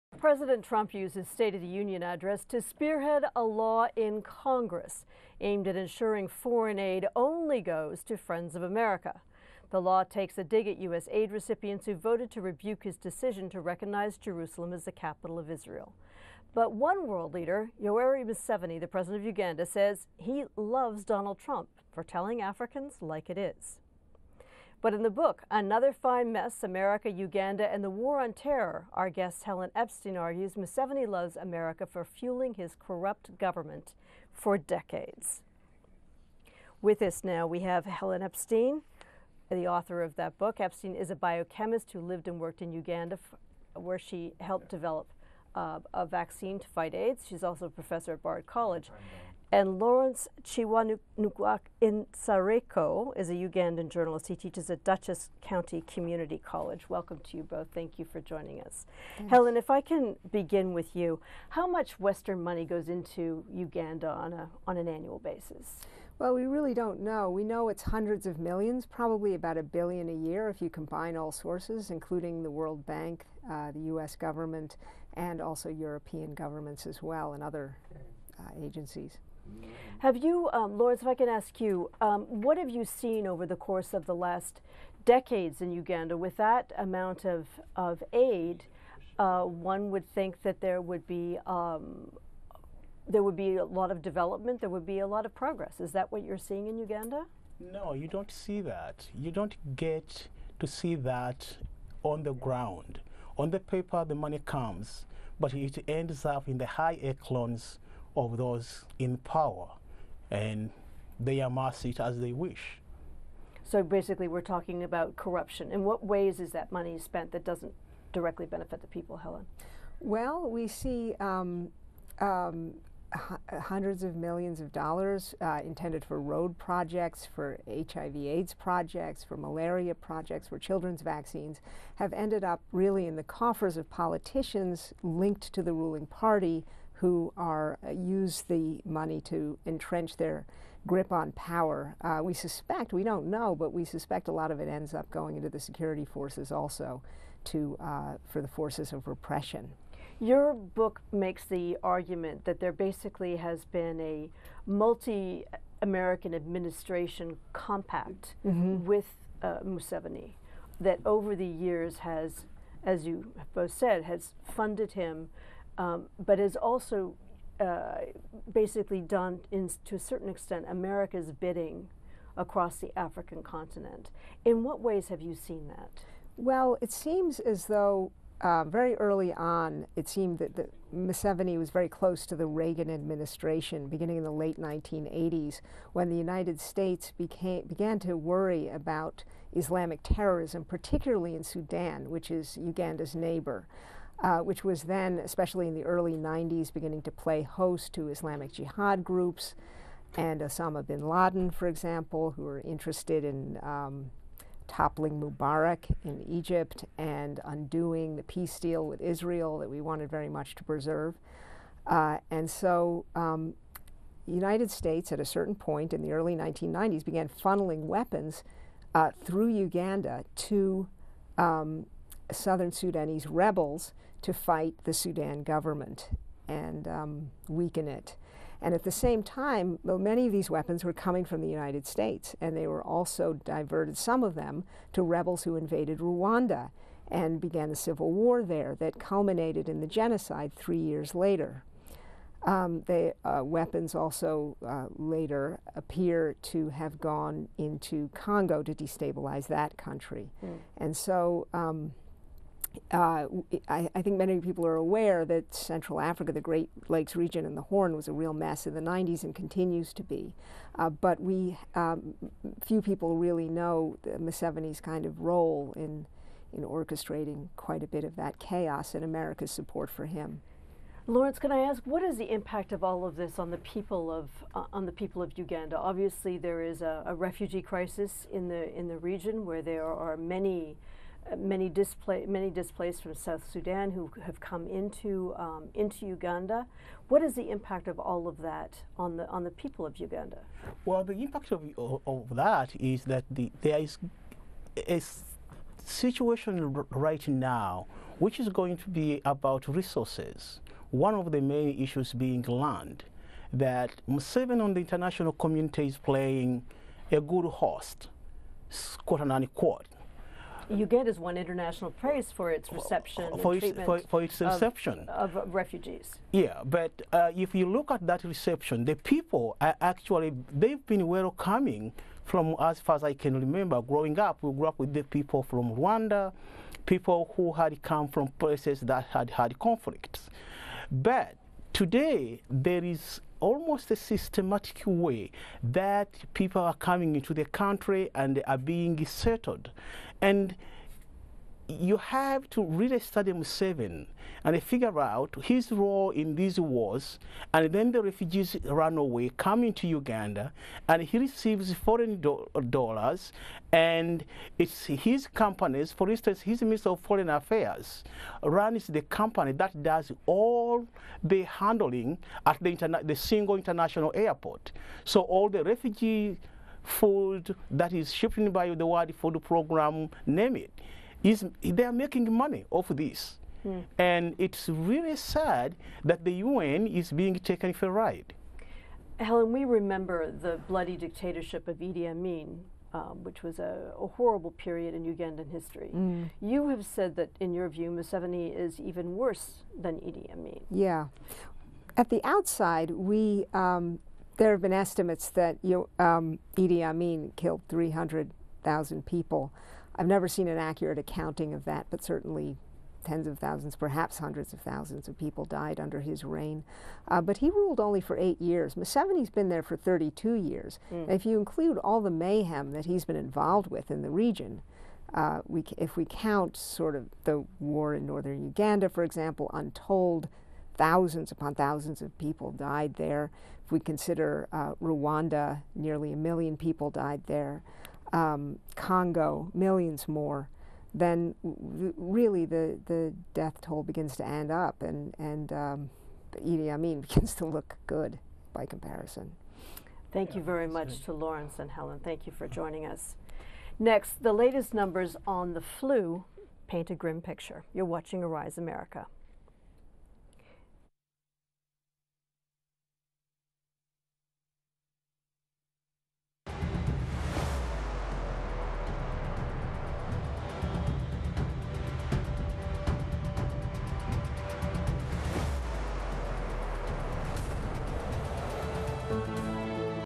Arise America: Did You Hear What Happened In Uganda?
In her book “Another Fine Mess: America, Uganda, and the War on Terror” author Helen Epstein lays out a case that accuses the USA of helping undermine a nation that was once one of Africa’s most stable and progressive countries.  ARISE NEWS spoke to Epstein and Ugandan journalist and professor Lawrence Kiwanuka Nsereko.